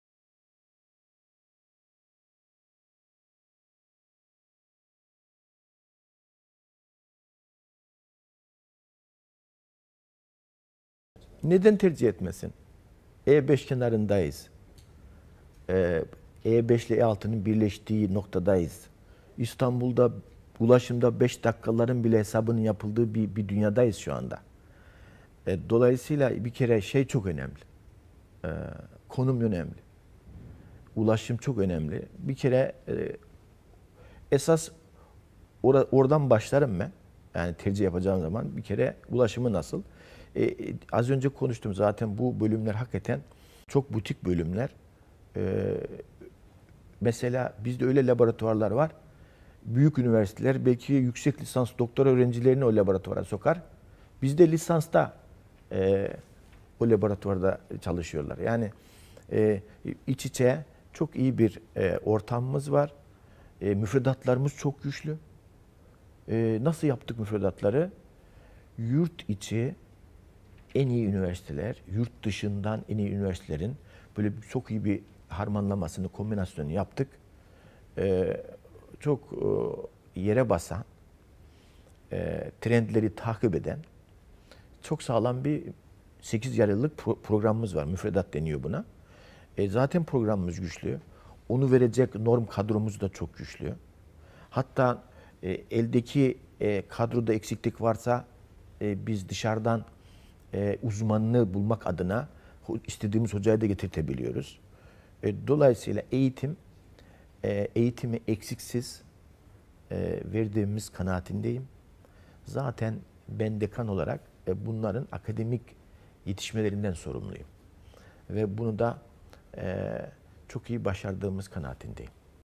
Öğrenciler Mühendislik Fakültesi'ni Neden Tercih Etmeli ?
İstanbul Ticaret Üniversitesi Mühendislik ve Tasarım Fakültesi Dekanı Prof. Dr. Muammer Kalyon öğrencilerin Mühendislik Fakültesi'ni neden tercih etmesi gerektiğini anlatıyor.